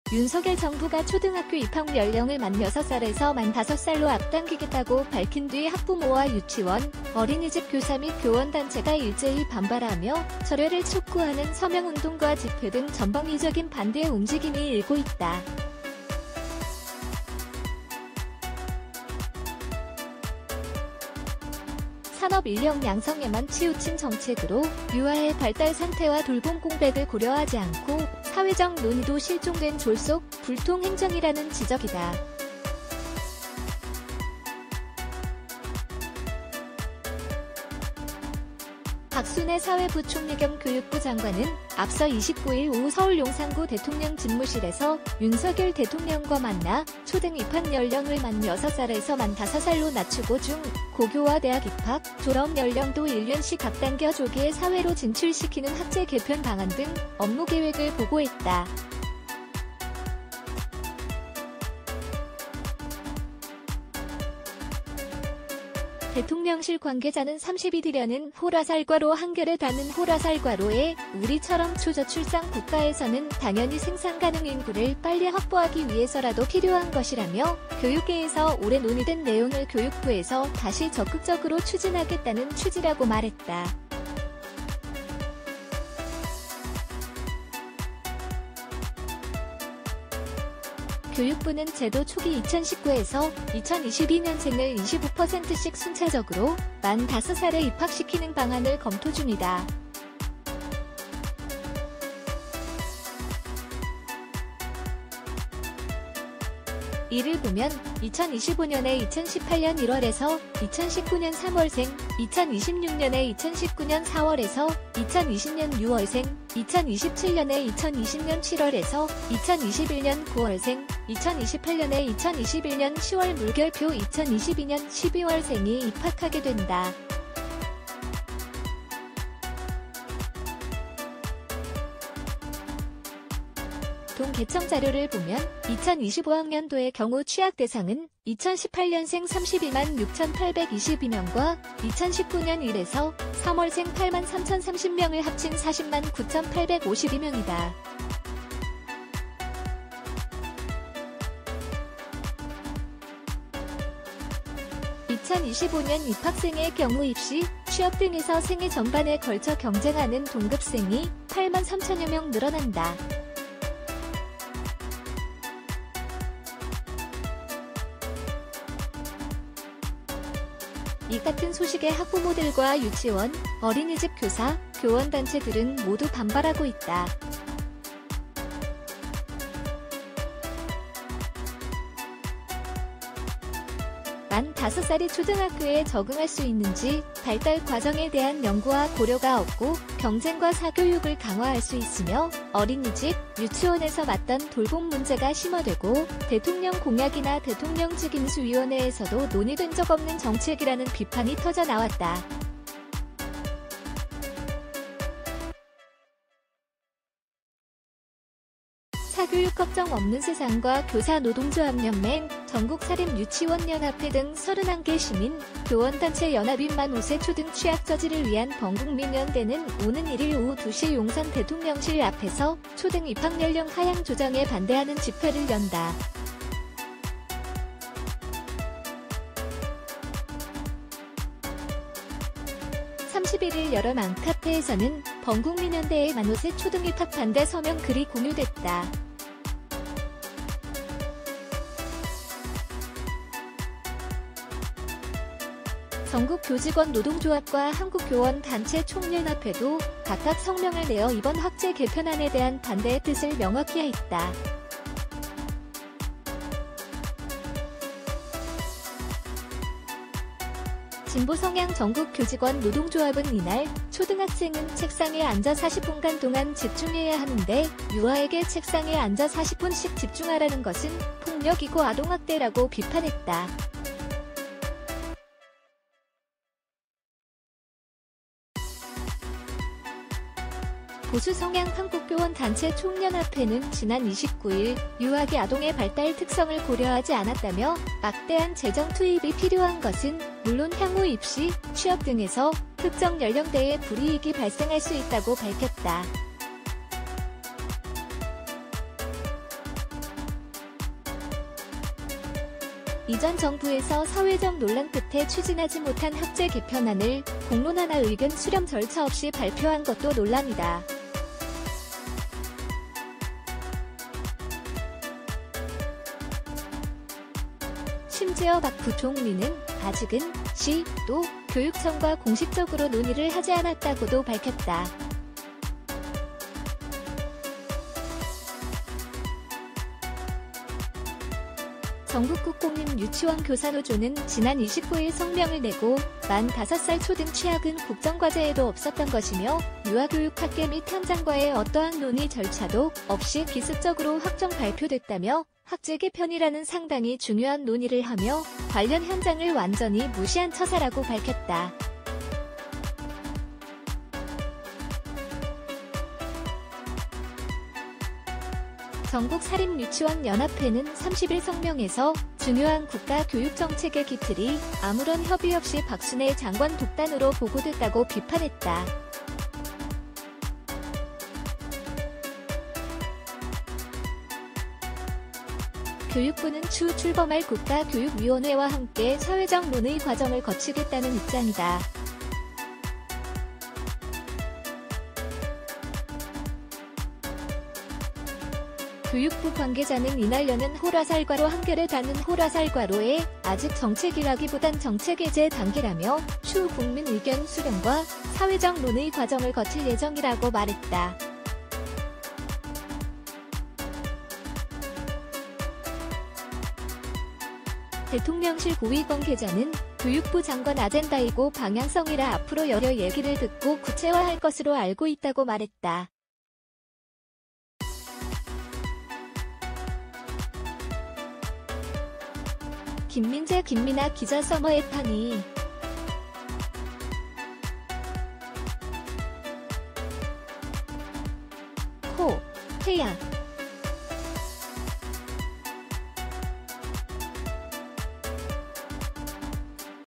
“만 5살에 40분 수업 강요, 폭력적!” 그 누구도 환영하지 않는 초등 입학연령 하향 추진, 결국 사람들은 결단을 내렸다
윤석열 정부가 초등학교 입학 연령을 만 6살에서 만 5살로 앞당기겠다고 밝힌 뒤 학부모와 유치원·어린이집 교사 및 교원단체가 일제히 반발하며, 철회를 촉구하는 서명운동과 집회 등 전방위적인 반대 움직임이 일고 있다. 산업 인력 양성에만 치우친 정책으로, 유아의 발달 상태와 돌봄 공백을 고려하지 않고, 사회적 논의도 실종된 졸속·불통 행정이라는 지적이다.    박순애 사회부총리 겸 교육부 장관은 앞서 29일 오후 서울 용산구 대통령 집무실에서 윤석열 대통령과 만나 초등 입학 연령을 만 6살에서 만 5살로 낮추고 중·고교와 대학 입학·졸업 연령도 1년씩 앞당겨 조기에 사회로 진출시키는 학제 개편 방안 등 업무계획을 보고했다. 대통령실 관계자는 31일 한겨레에 “우리처럼 초저출산 국가에서는 당연히 생산 가능 인구를 빨리 확보하기 위해서라도 필요한 것”이라며 “교육계에서 오래 논의된 내용을 교육부에서 다시 적극적으로 추진하겠다는 취지”라고 말했다.    교육부는 제도 초기 2019~2